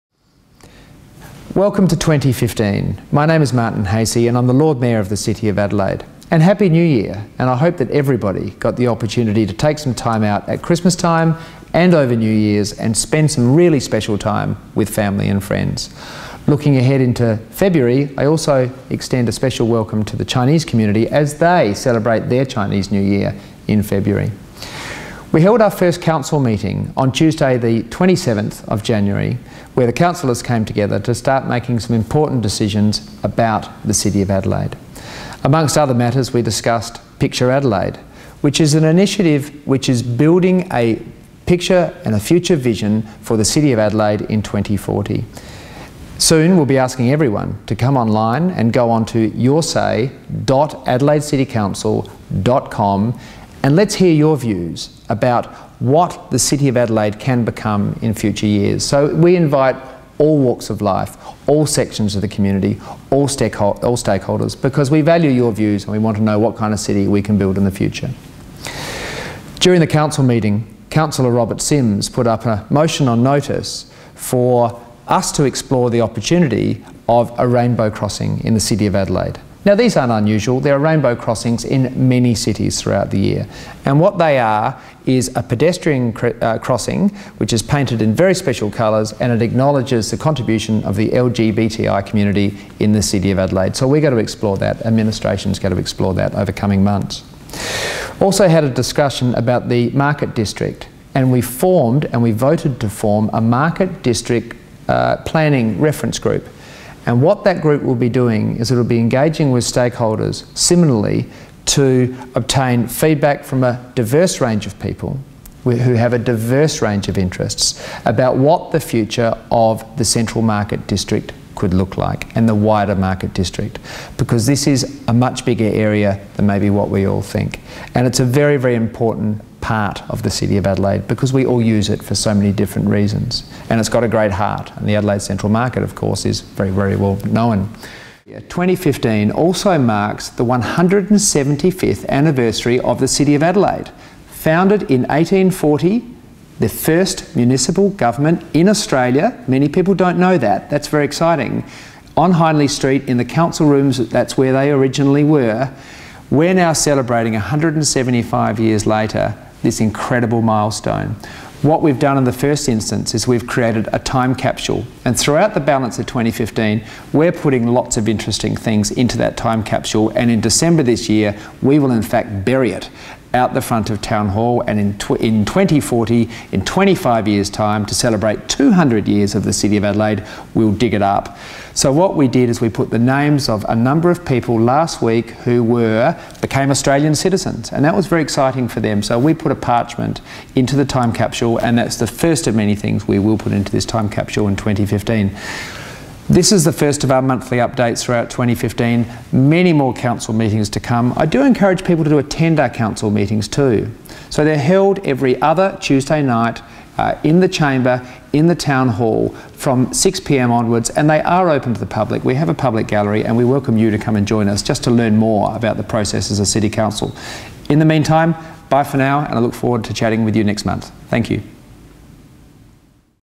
Council Updates - January 2015
Lord Mayor Martin Haese delivers his first Adelaide City Council monthly meeting update. January includes: what's coming for Picture Adelaide 2040, Councillor Simms' Rainbow Crossing proposal, the future of the Market District, and 175 years of the City of Adelaide.